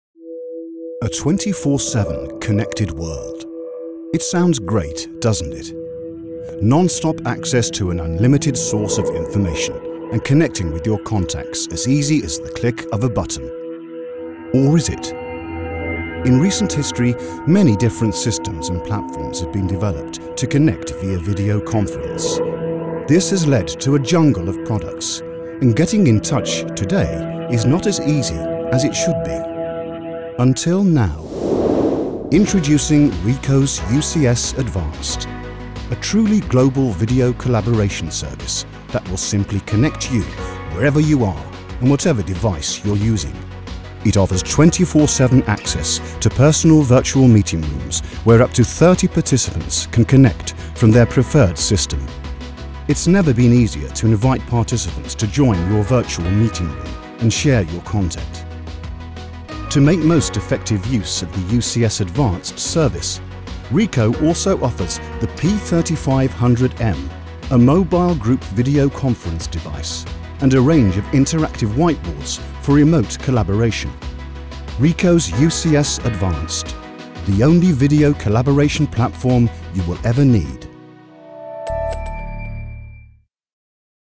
Ricoh UCS Advanced - The only video collaboration platform you will ever need...
Ricoh's Unified Communication System (UCS) Advanced is a truly global video collaboration service that assures interoperability using a Virtual Meeting Room (VMR). The online meeting space that can be accessed from a diverse range of devices and communication platforms. It is fully compatible with WebRTC and most common audio/video conferencing end points, it is available 24/7 and can accommodate up to 30 participants.